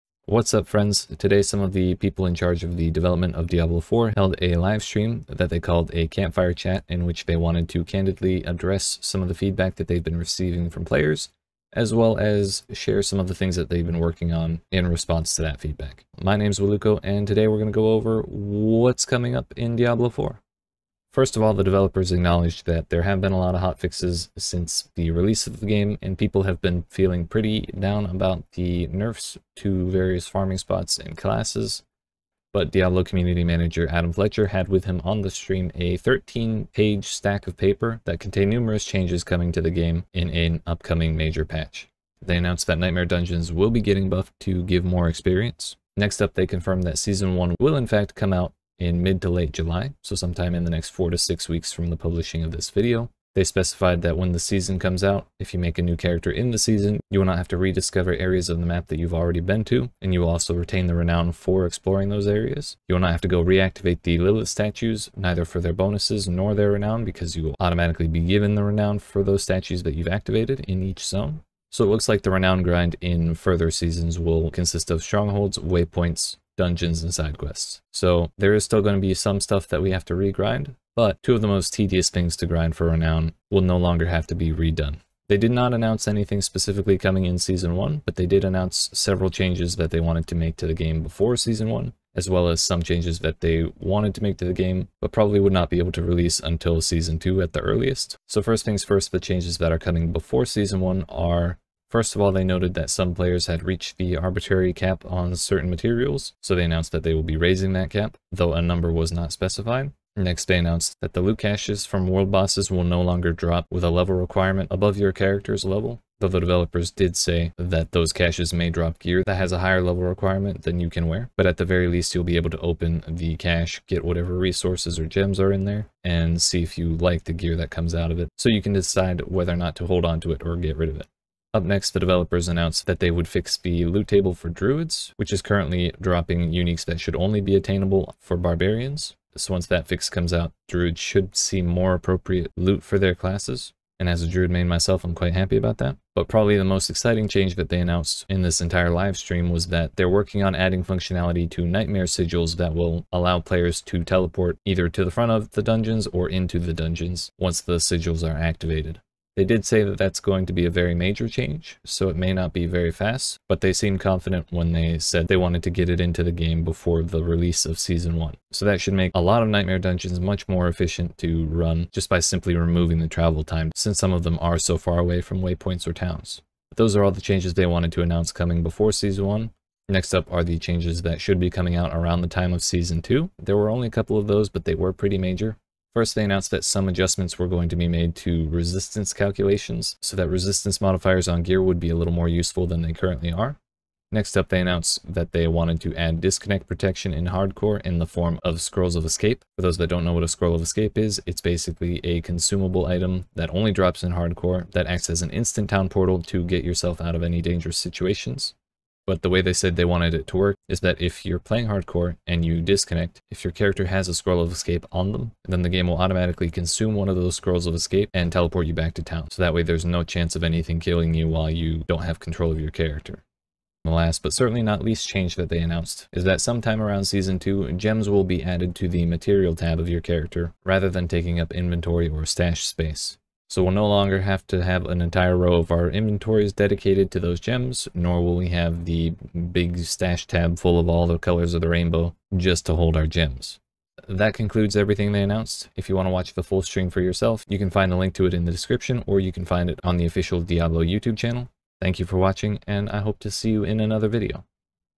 GEMS IN THE MATERIAL TAB?? BUFFS COMING SOON?? | Diablo 4 Campfire Chat Dev Stream Recap
Yesterday some of the people in charge of the development of Diablo 4 held a livestream in which they spoke candidly about some of the feedback that the community has been giving about D4 as well as some of the things they're working on to address many of the community's concerns. They talked about a lot of cool things on the way and I quickly break them all down in this video!